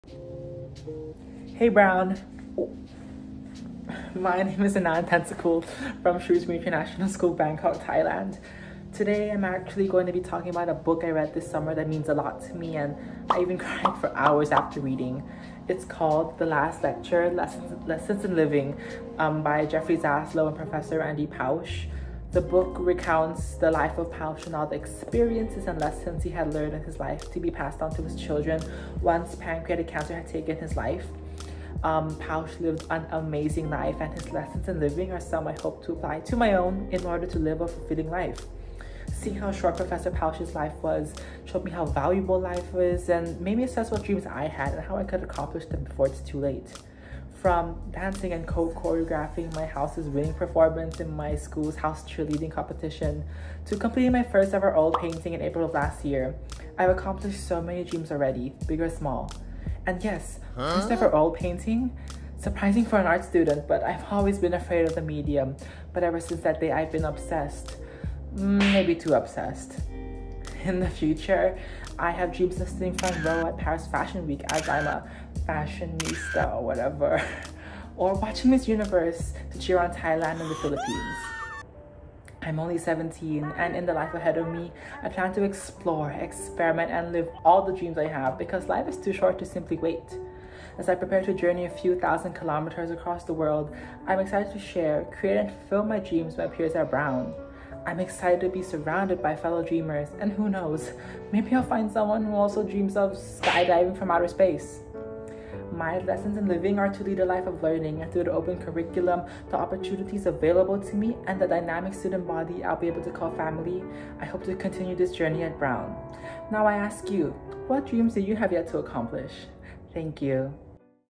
ACCEPTED brown university video portfolio (class of 2026!!)
hey everyone!! it's anand this time, but i just wanted to share the video portfolio that i submitted for my application to brown university, early decision. it's definitely not the best video out there, and is not what (i think) got me accepted but i hope this can help inspire prospective brown students with their video ideas and brainstorming!! i literally just sat in front of my iphone and talked about a book lol. but yes, i really hope you all enjoy it and i can't wait to be spending the next 4 years of my life at brown :) and lmk if you'd like more videos about brown, the application process or anything else!!